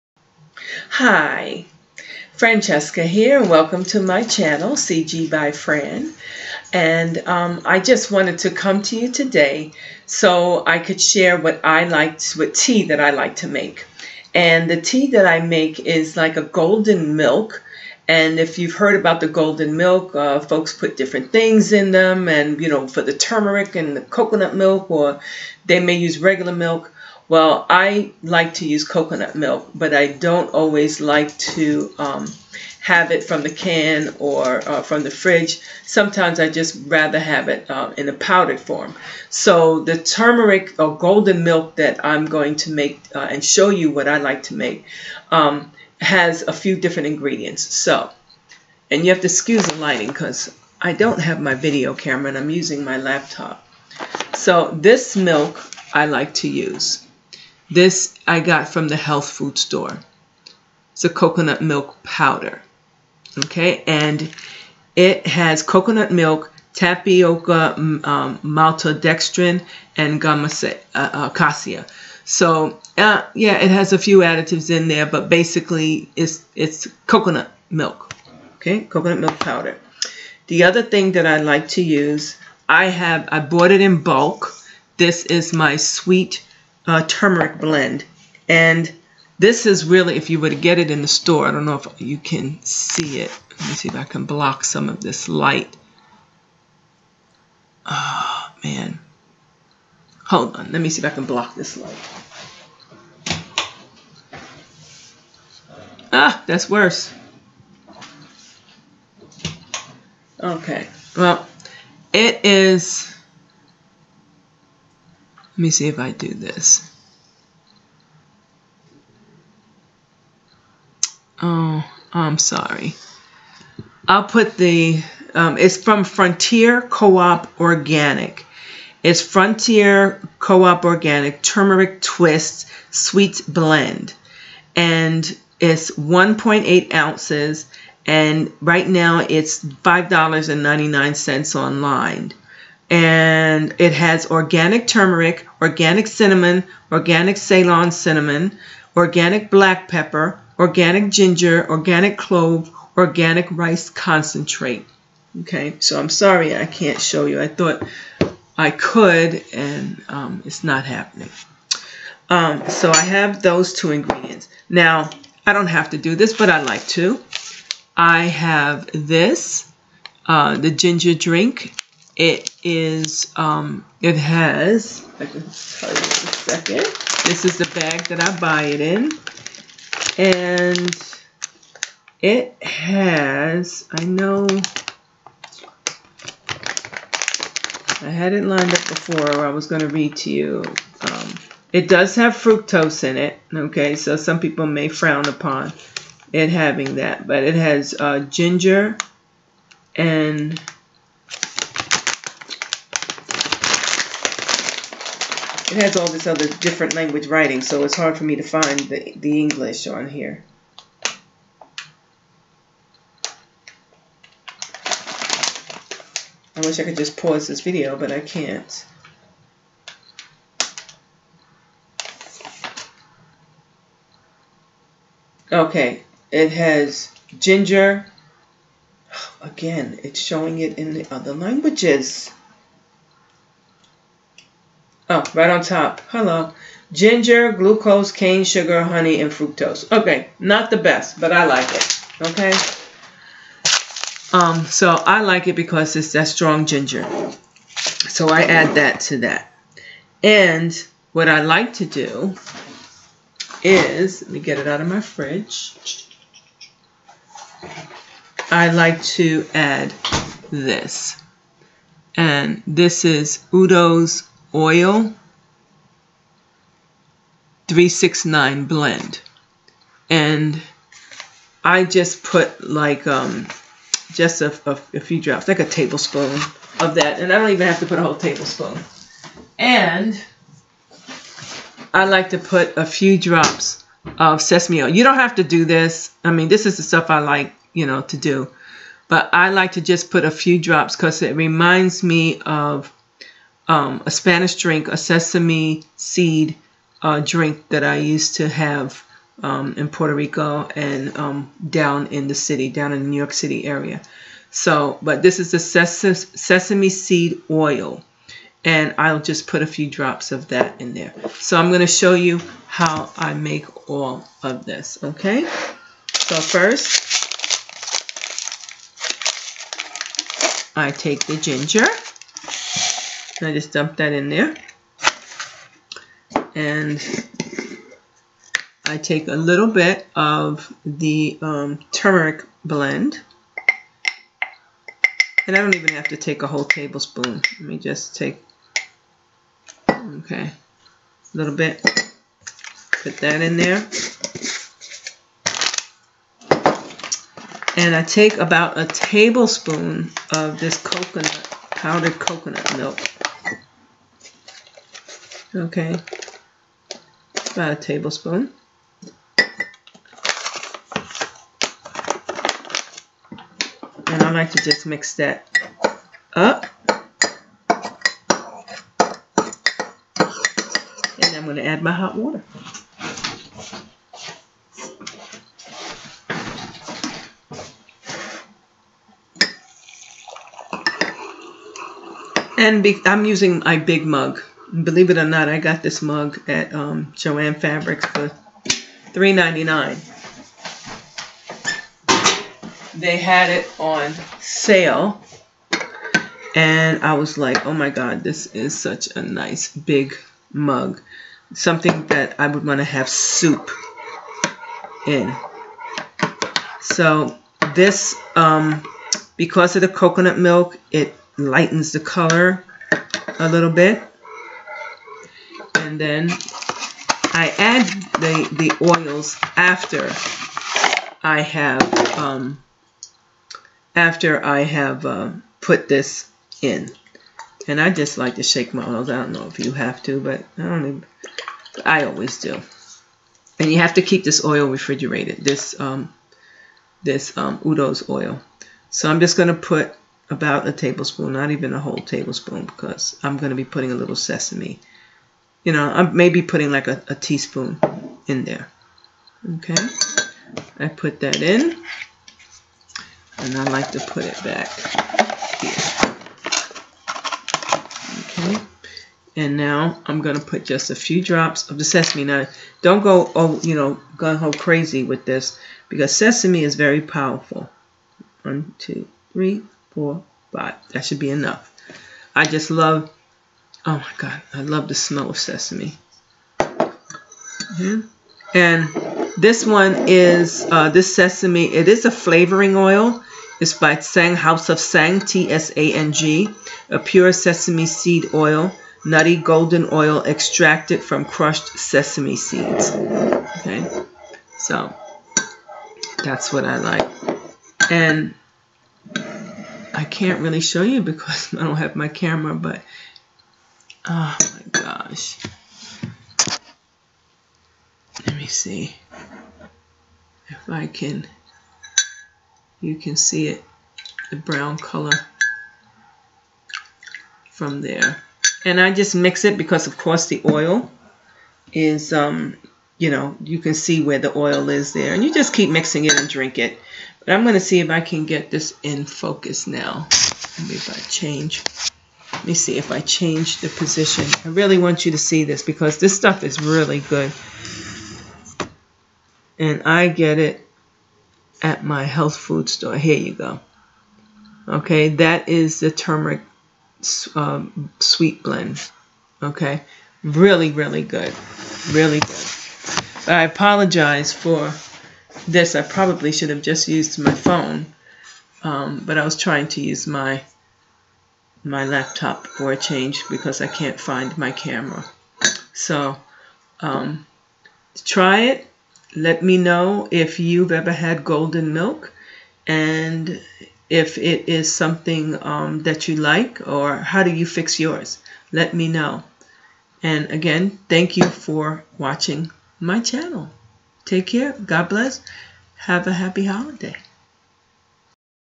Golden Milk - #2
I show you what I use to make my Golden Milk.

I was unable to edit this file because I used my laptop instead of my video camera.  I apologize in advance for the few gaps when I'm trying to get info.

I purchased the:
- Frontier Co-op Organic turmeric twist sweet blend, the Native Forest Coconut Milk Powder, and the Udo's 3-6-9 Oil from my local health food store (in bulk).

- Gold Kili Ginger Drink from my local West Indian food market.  You should also be able to get it at a local Asian market.

-House of Tsang Sesame Seed Flavoring Oil from my local grocery store.

All of the above can be purchased online, as well.